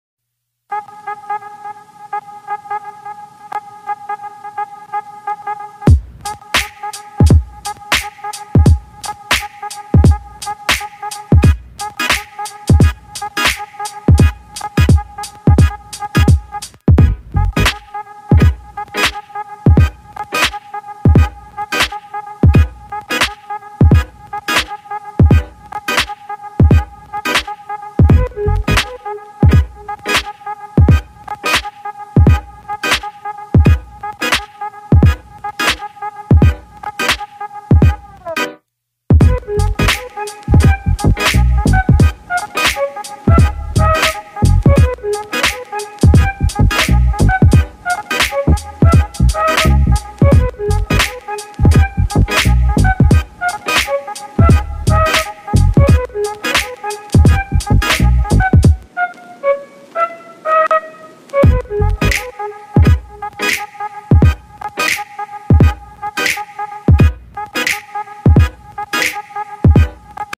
Just a normal day but Oliver's going out with Felix|| skit || read desc please
So if you don't know G/me is pregnant but here is the future with that's why Oliver looks older and if ur asking who's the girl that looks like me that is mah future daughter but that's in the future and here in the present they don't know what gender the baby is yet so yea ✌ thanks for reading this :P :3